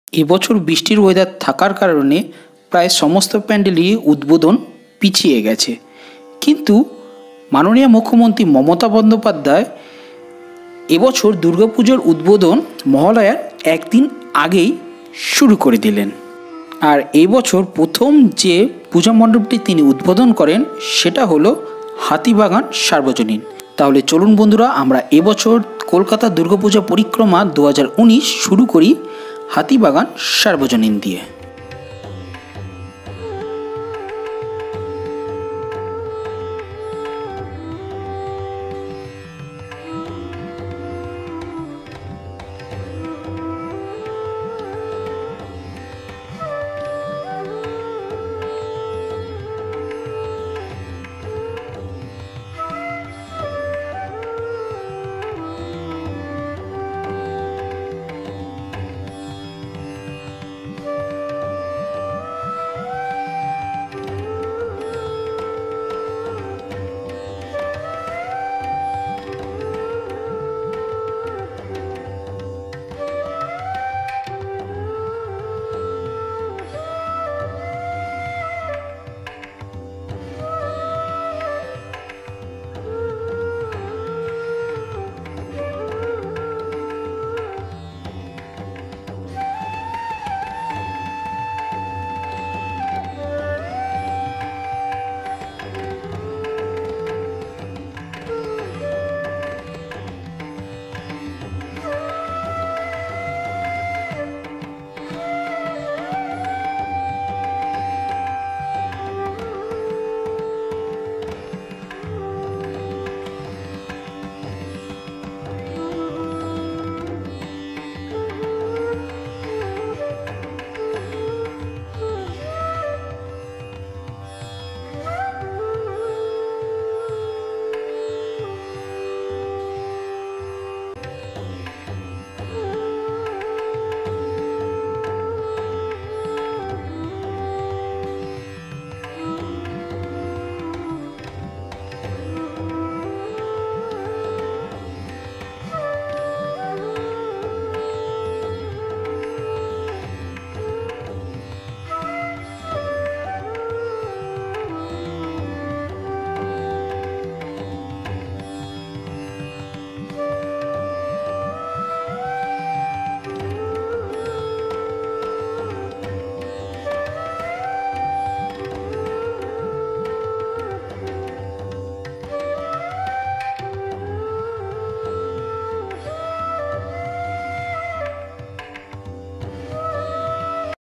Durga Puja 2019 Kolkata | Hatibagan Sarbojanin Durga Puja 2019 | Durga Pujo 2019 Theme Pandal
Durga Puja 2019 at Kolkata. Durga Puja 2019 at Hatibagan Sarbojanin in Kolkata, West Bengal, India. The Durga Puja 2019 festival has sarted with the innauguration of Hatibagan Sarbojanin Durga Puja 2019 pandal by West Begal Chief Minister Srimati Mamta Banerjee. After popular theme of Durga Puja 2014, Durga Puja 2015, Durga Puja 2016, Durga 2017 and Durga Puja 2018 - what is the next surpise from Hatibagan Sarbojanin Durga Puja committee? In 2019 Durga Puja their theme is "Traditional Chalchitro Art". Here is the Durga puja 2019 theme pandal video of Hatibagan Sarbojanin at Hatibagan area in North Kolkata, West Bengal in India. This pandal can be reached from Sovabazar metro rail station by auto or bus or by walk.